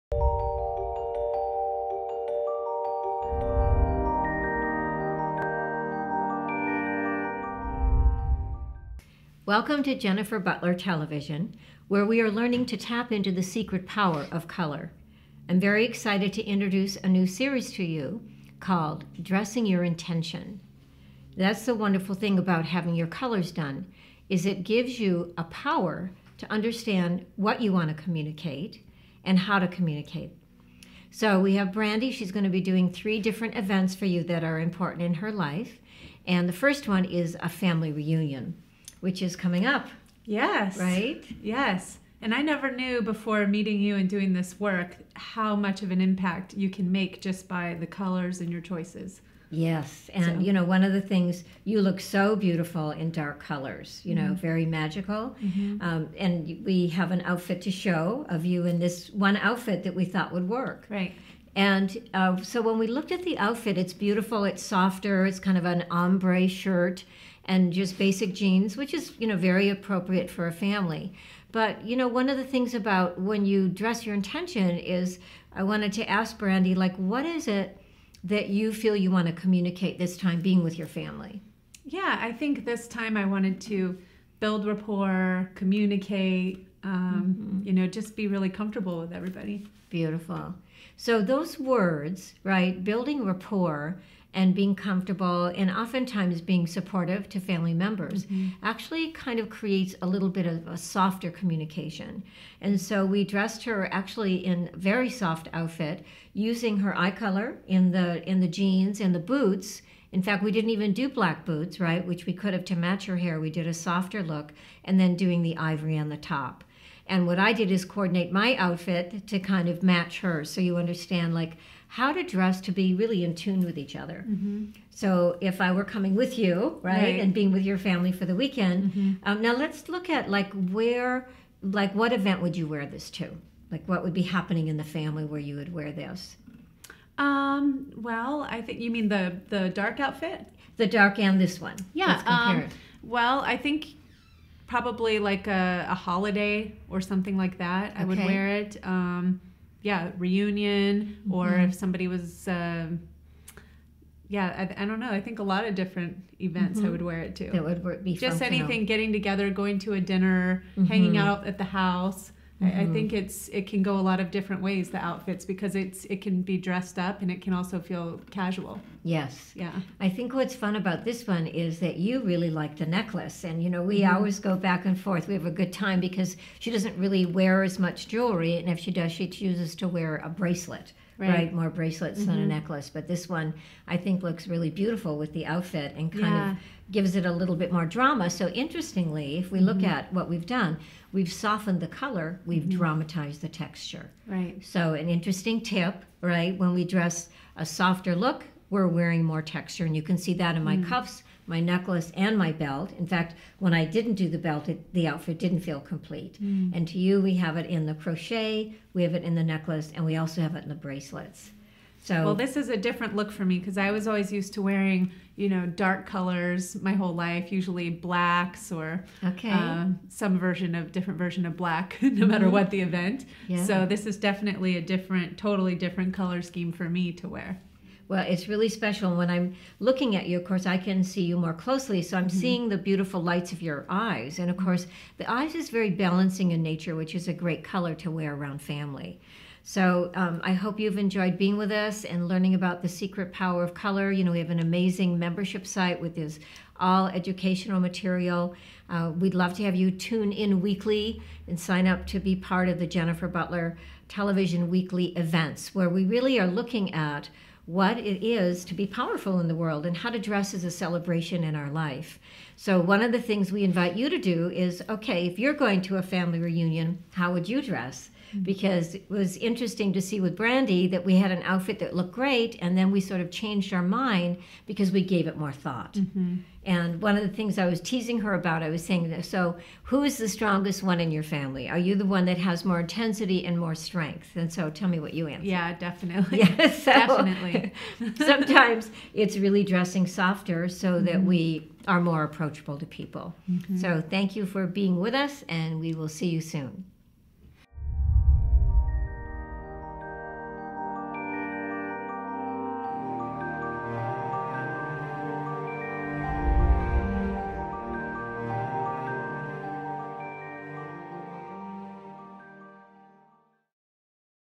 Learn How to Dress with Intention for a Family Reunion
Family reunions are an opportunity to reconnect, or bond with new family members. What lasting impression do you want to create? Learn how to dress with intention for a family reunion.